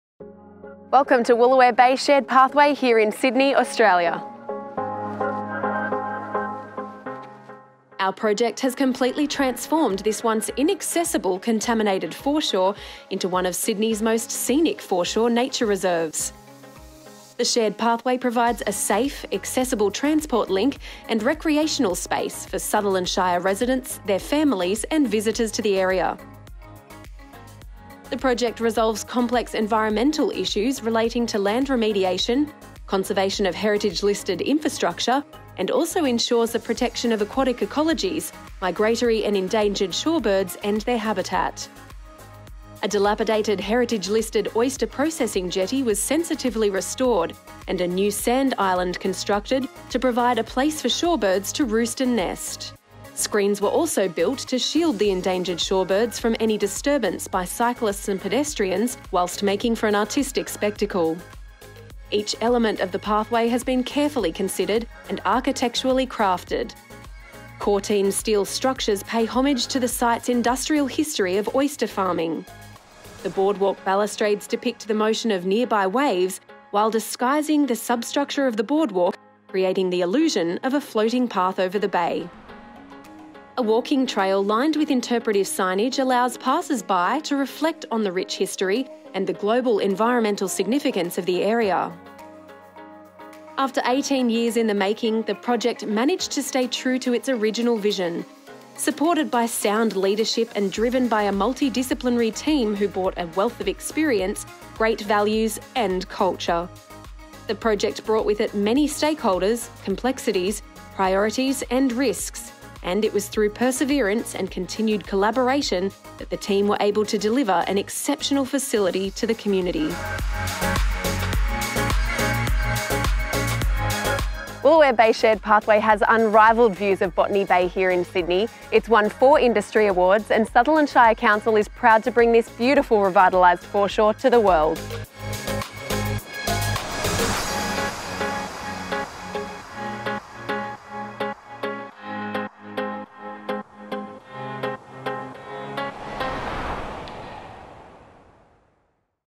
Finalist PE Award 2019: Woolooware Bay Shared Pathway Stage 6, Taren Point, Australia
by Sutherland Shire Council in the category Social/Regional Development/Community Service